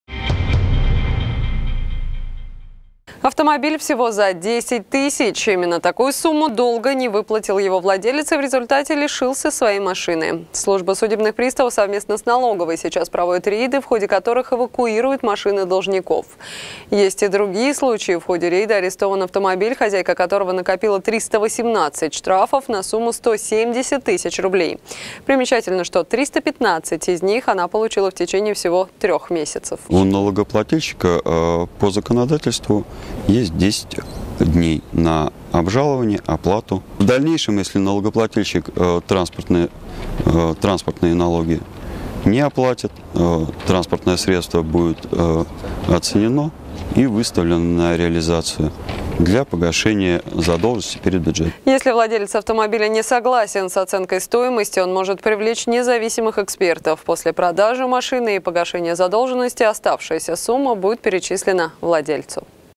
UTV. У уфимки арестовали автомобиль за долг в 10 тысяч рублей
Автомобиль всего за 10 тысяч. Именно такую сумму долга не выплатил его владелец, и в результате лишился своей машины. Служба судебных приставов совместно с налоговой сейчас проводят рейды, в ходе которых эвакуируют машины должников. Есть и другие случаи. В ходе рейда арестован автомобиль, хозяйка которого накопила 318 штрафов на сумму 170 тысяч рублей. Примечательно, что 315 из них она получила в течение всего трех месяцев.

Если владелец автомобиля не согласен с оценкой стоимости, он может привлечь независимых экспертов. После продажи машины и погашения задолженности, оставшаяся сумма будет перечислена владельцу.